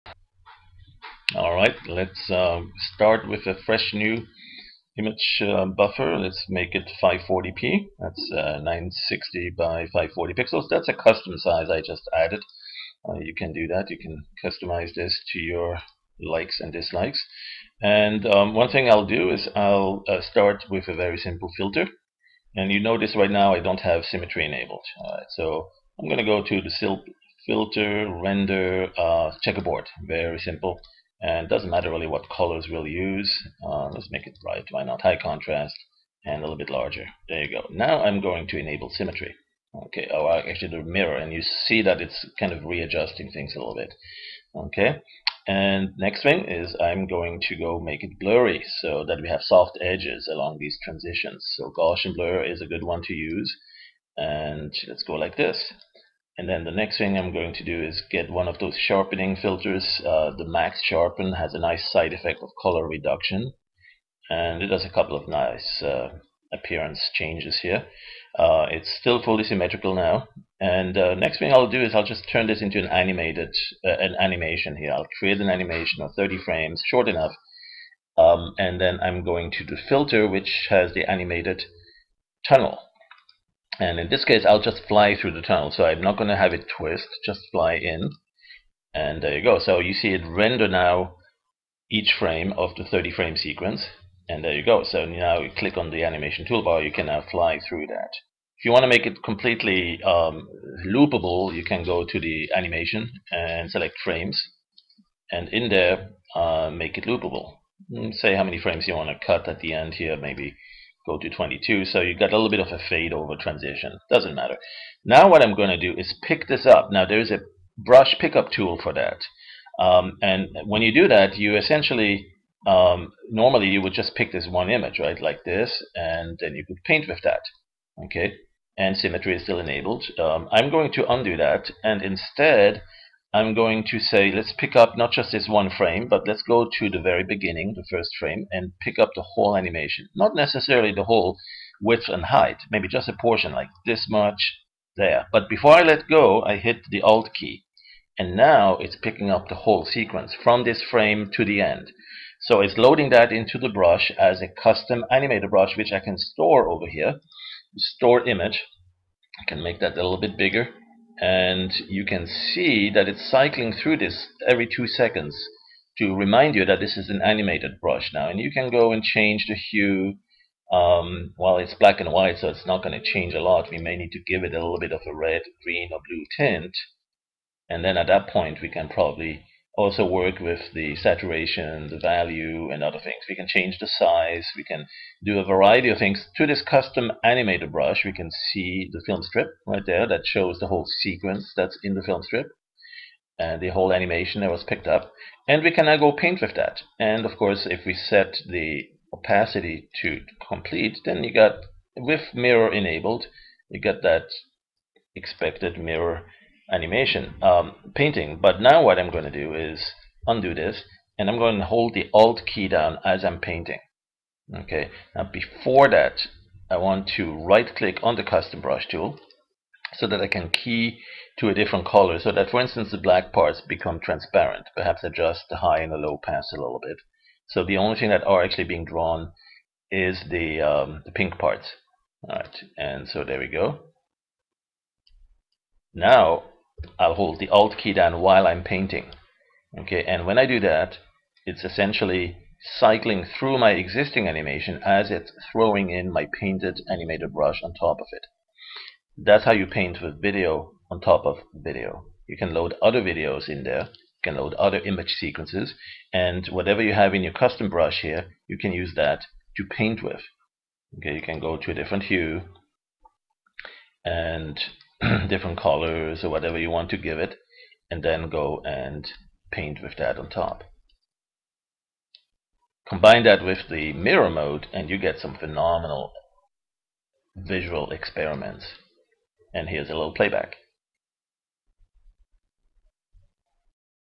brushes and mirrors and animated textures (oh my)
it's all coming together very nicely with PD Pro 5.1, a free update patch for users of PD Pro 5. Make image-based animated brushes, paint with video over video, and keep mirror mode enabled for perfectly symmetrical results in seconds.  This is great stuff for DJs and VJs